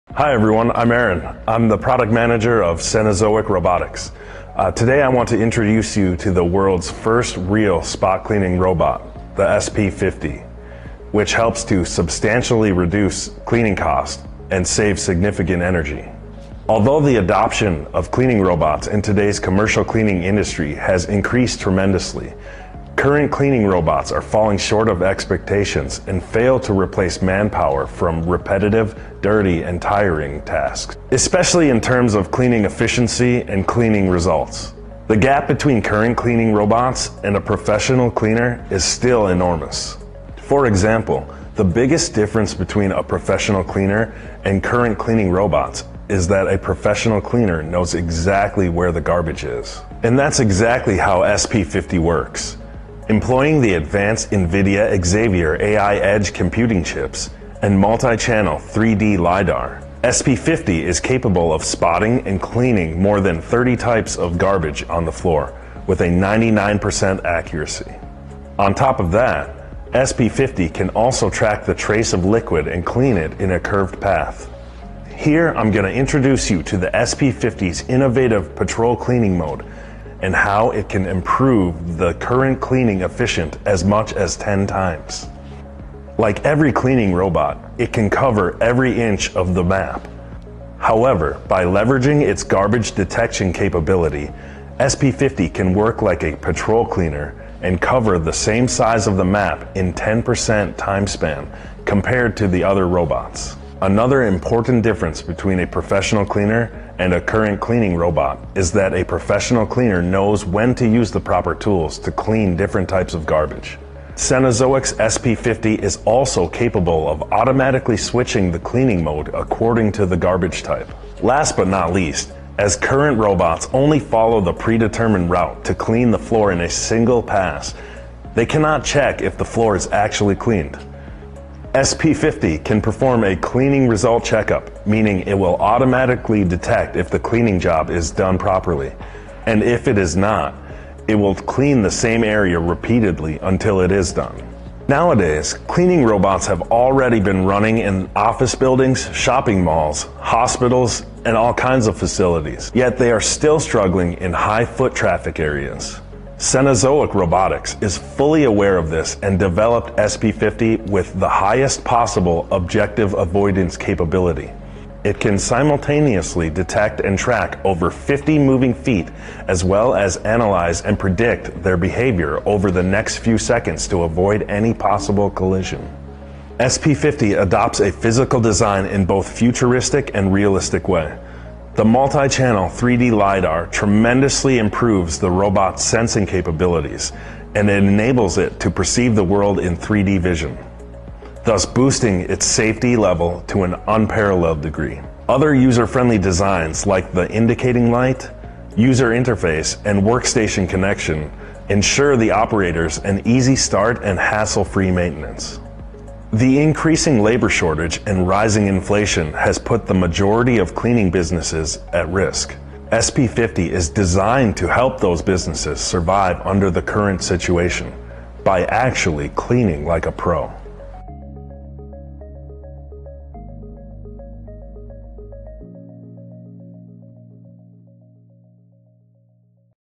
SP50, Spot-Cleaning Robot
Meet SP50, the groundbreaking spot-cleaning robot, setting new benchmarks of effectiveness and efficiency in the market today. With a remarkable capability of detecting over 30 different types of waste and debris with an astonishing 99% accuracy, this robot redefines intelligent cleaning. Discover the future of cleaning technology with the unparalleled performance of SP50.